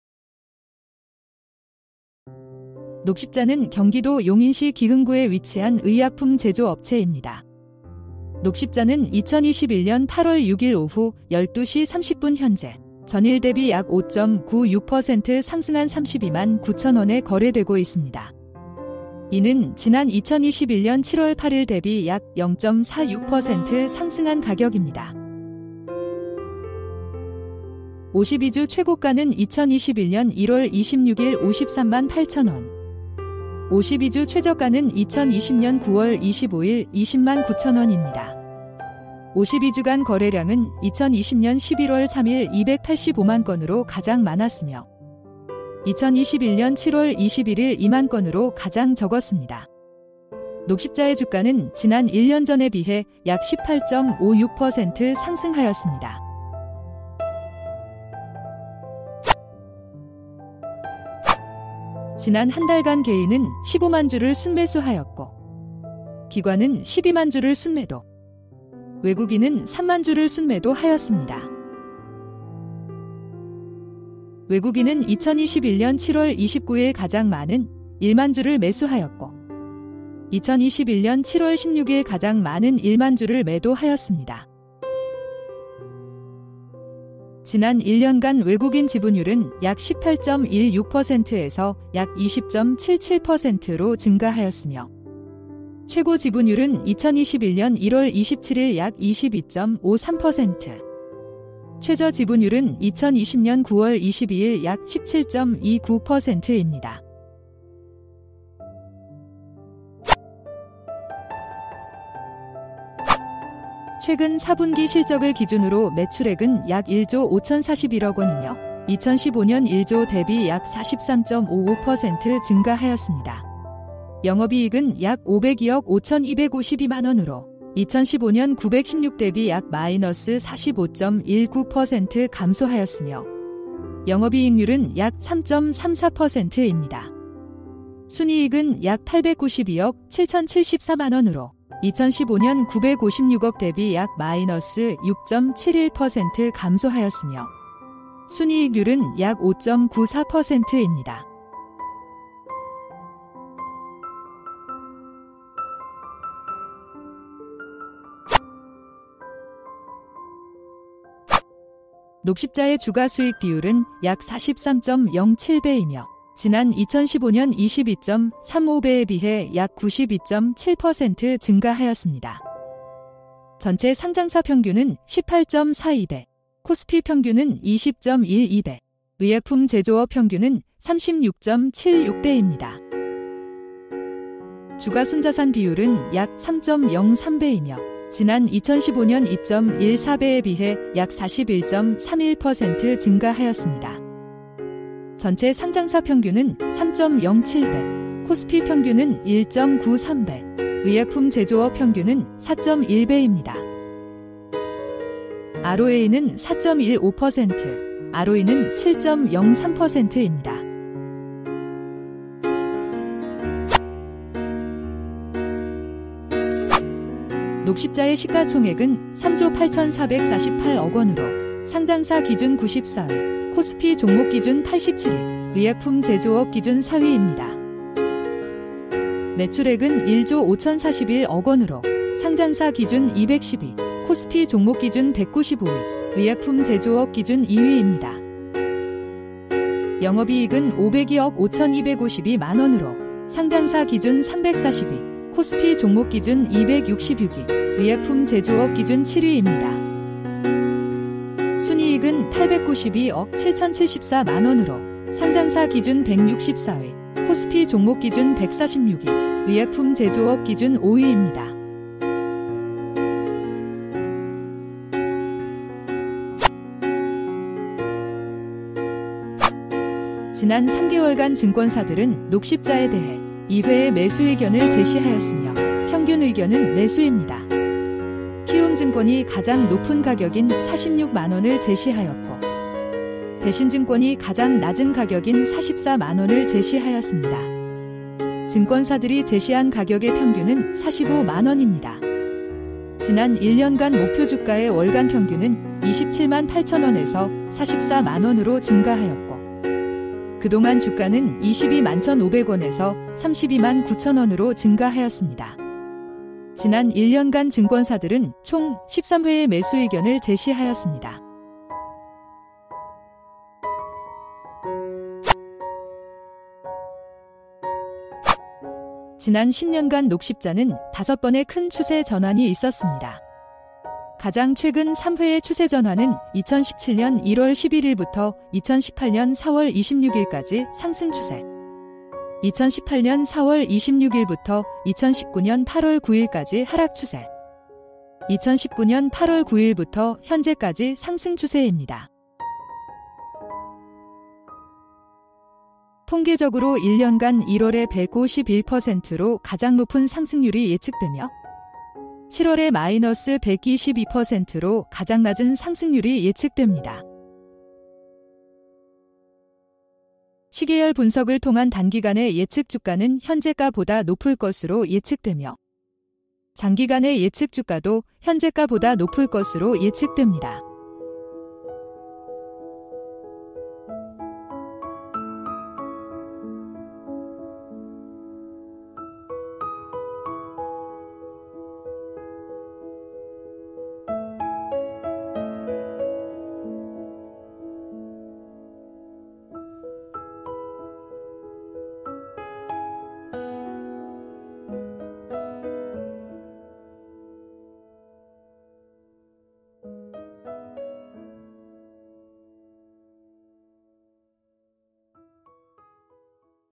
녹십자 주가분석006280 2021년 08월 06일기준
녹십자는 경기도 용인시 기흥구에 위치한 의약품 제조업체 입니다.
녹십자는 2021년 08월 06일 오후 12시 30분 현재